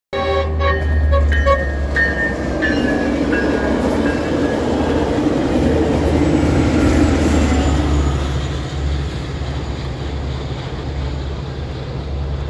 Metrolink 753 west Perris to Los Angeles. Horn and bell salute, just west of Fullerton Station.
This train originates in Perris, California traveling west to Los Angeles on the BNSF tracks. We usually catch them in Riverside.  We guessed they saw that familiar red truck and said hi.